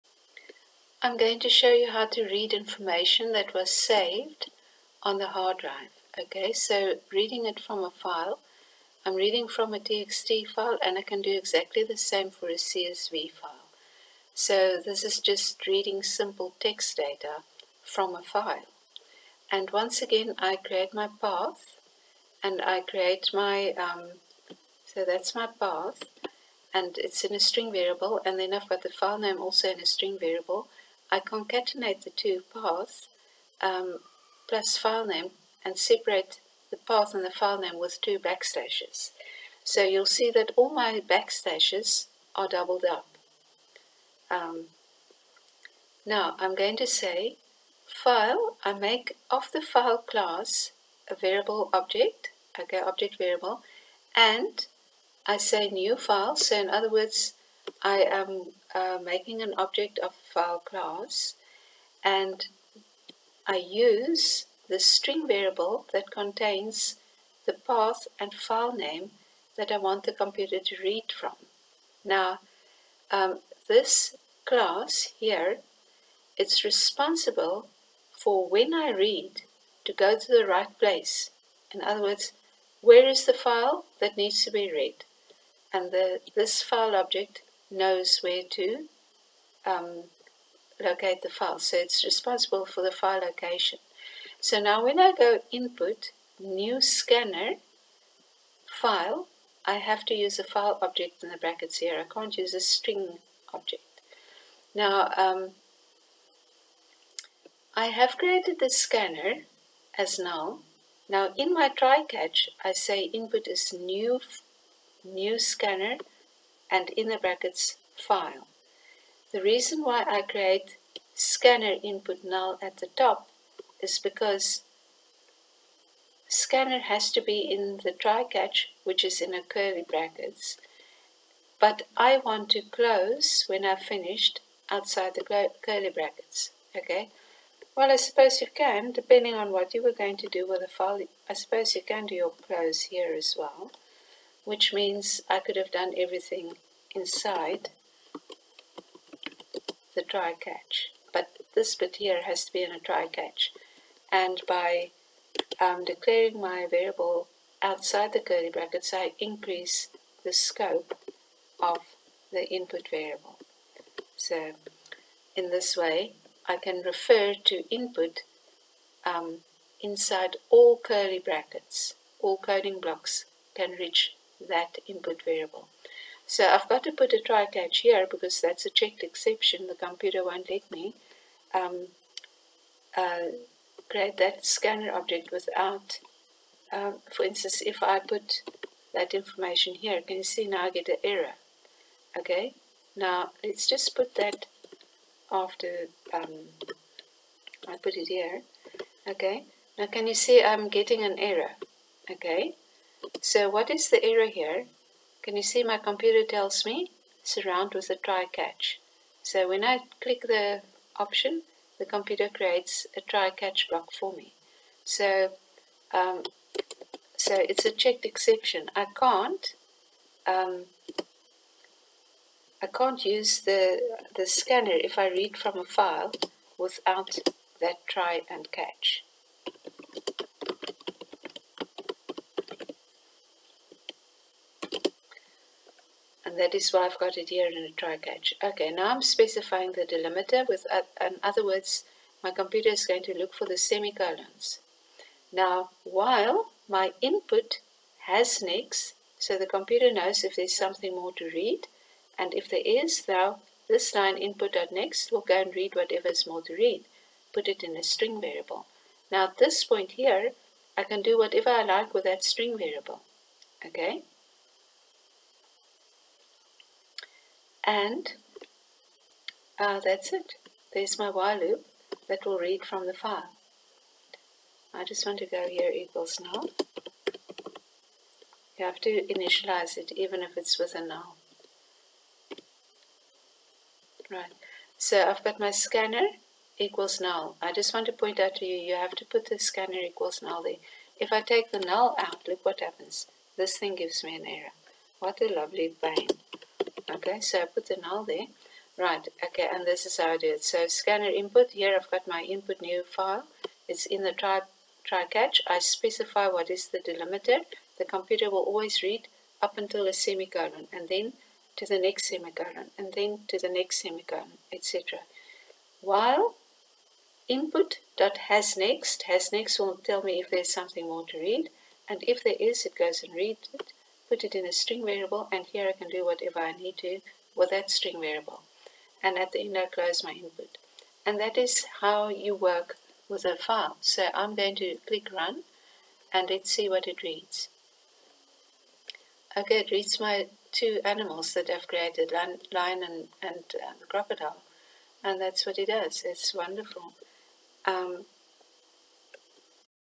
Java Files 3: Read csv or txt data using the Scanner
Java Files 3: Read csv or txt data using the Scanner , do the full course